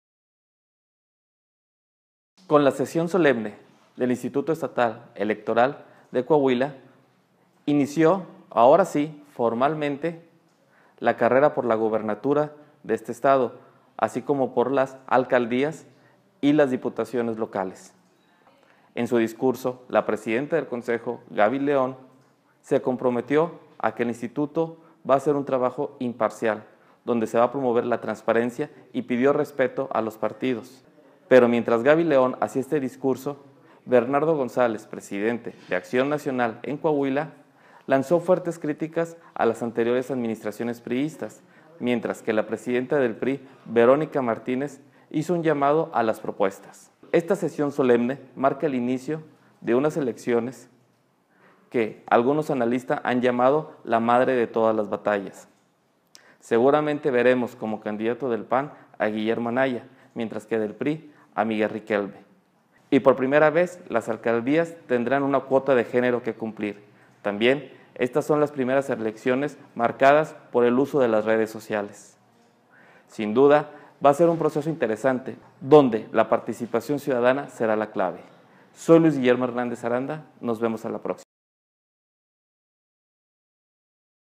Arranca carrera por la gubernatura
Con la instalación de la sesión solemne del Instituto Electoral de Coahuila,​ celebrada este martes en el Museo del Desierto de Saltillo, inició formalmente el proceso electoral que culminará el primer domingo de junio con la elección de gobernador, alcaldías y diputados locales.